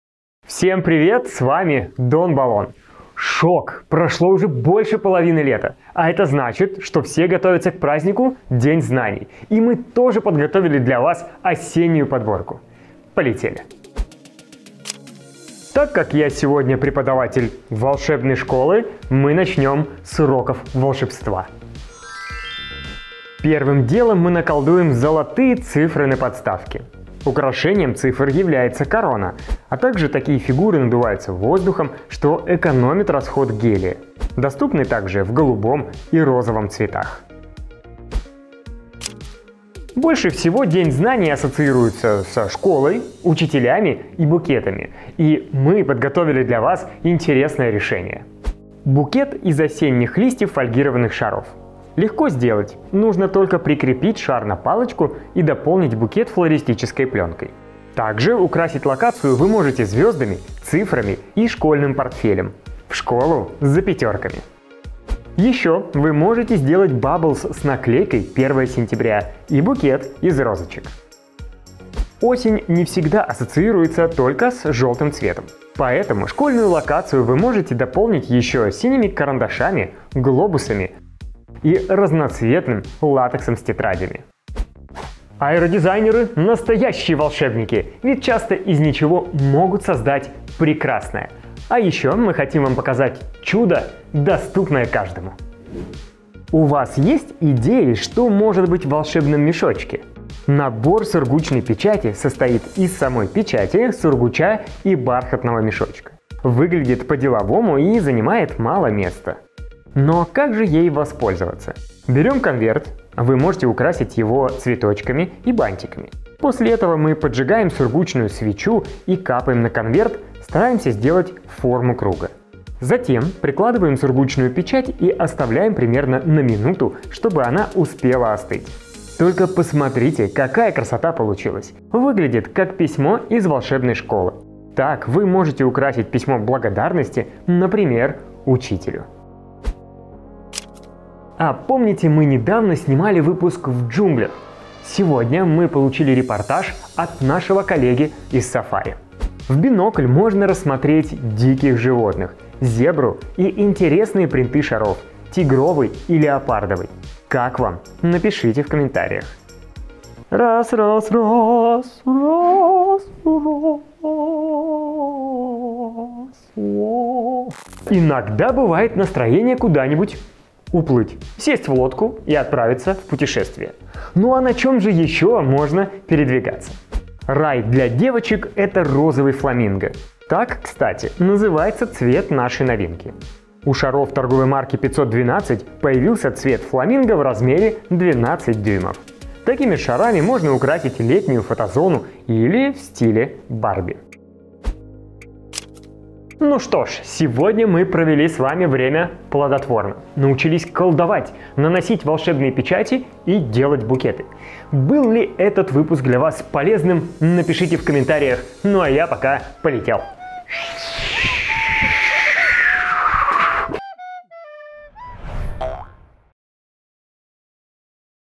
Новости: Готовимся к школе с магистром - ПОДБОРКА ИДЕЙ И ВДОХНОВЕНИЕ
Таймкоды:
00:00 Вступление
00:22 Новые цифры
00:39 Подборка к школе
01:24 Набор сургуч и штамп
02:19 Сафари
02:47 Новый цвет 512
03:16 Завершение